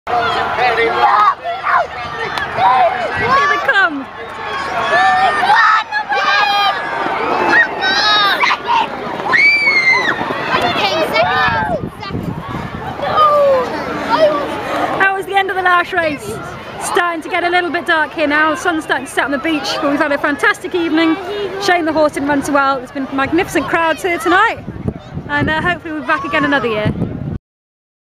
Laytown Beach Horse Racing.
We watch the last race of the day at Laytown Strand races, where horses race on the beach in Ireland.